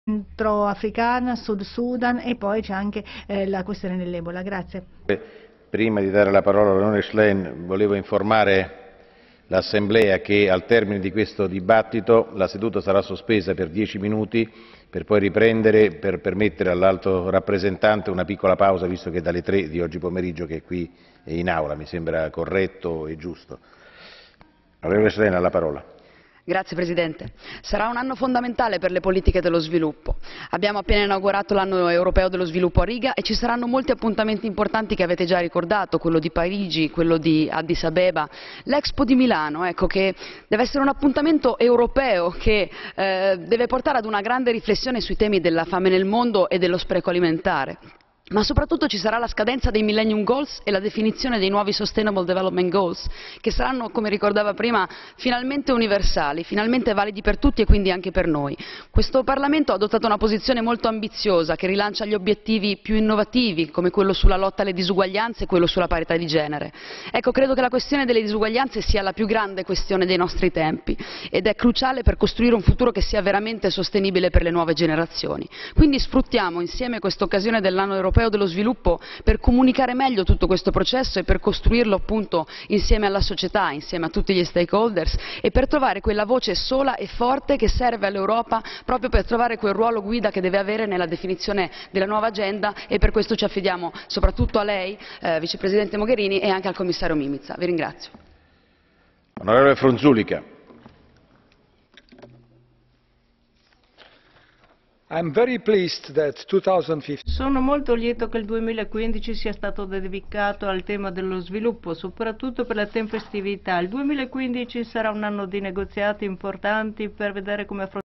Schlein - anno europeo dello svilupo 14 01 14
Intervento in aula di Elly chlein sul lancio dell'anno europeo dello sviluppo